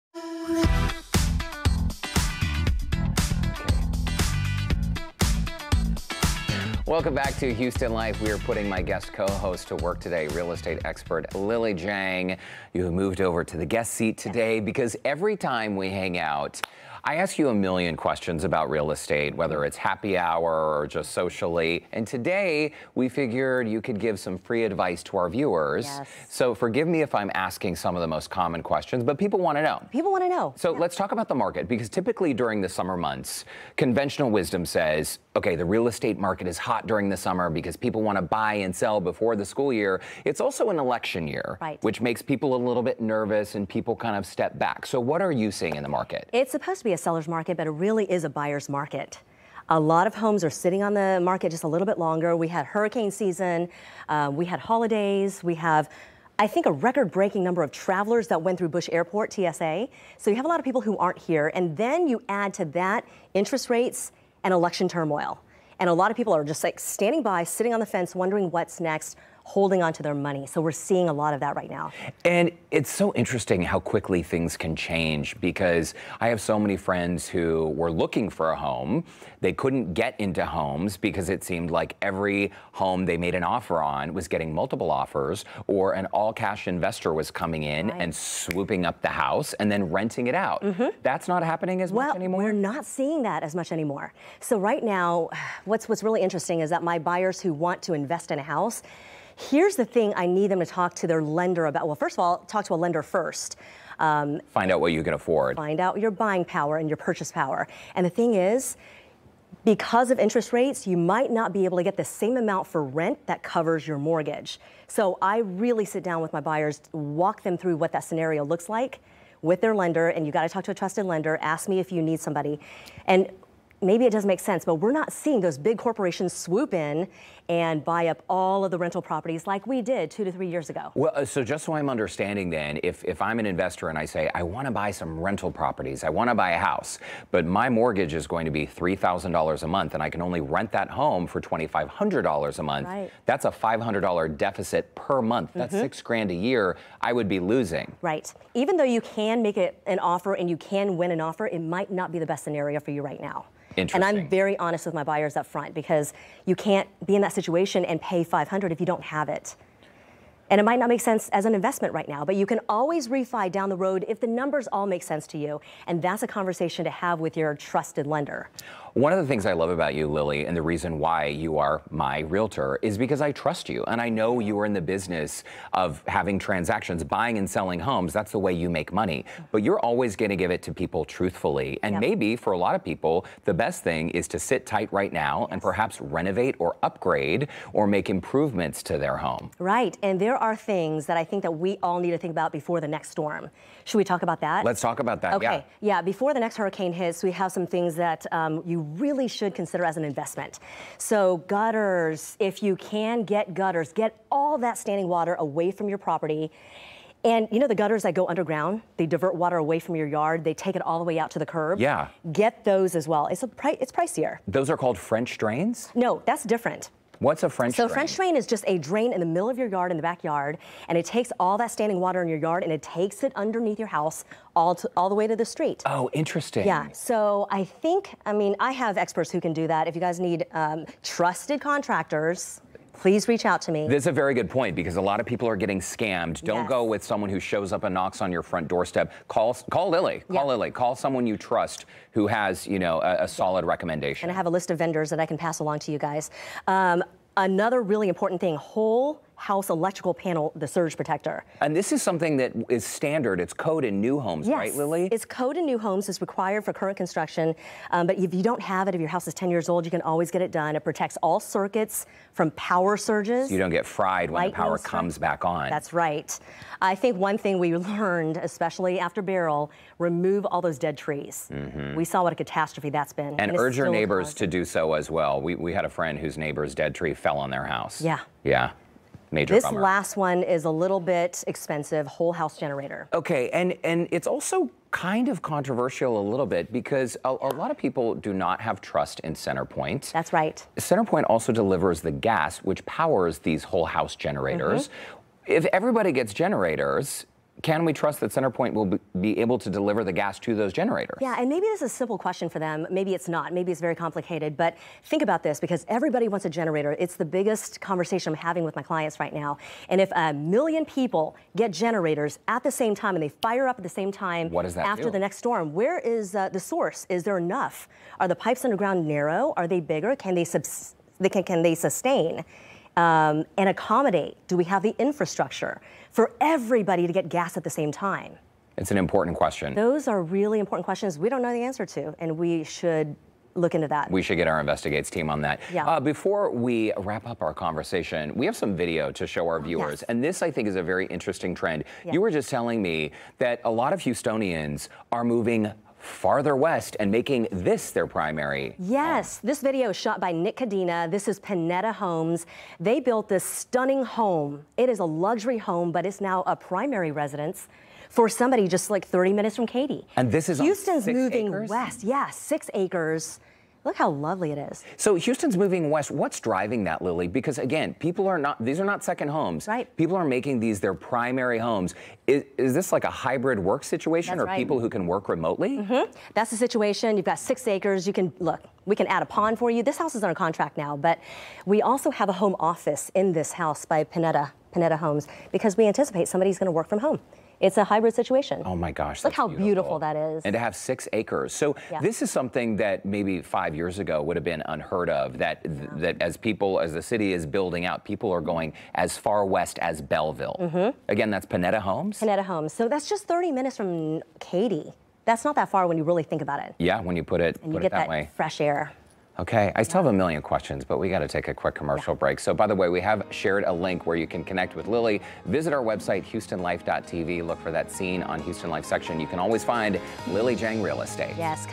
Real estate expert Lily Jang breaks down the latest trends in the Houston housing market
Whether you're considering buying, selling, or renovating a home, timing can be everything.  Real estate expert Lily Jang shares the latest on the Houston housing market and offers advice for getting your home prepared for the next major weather event.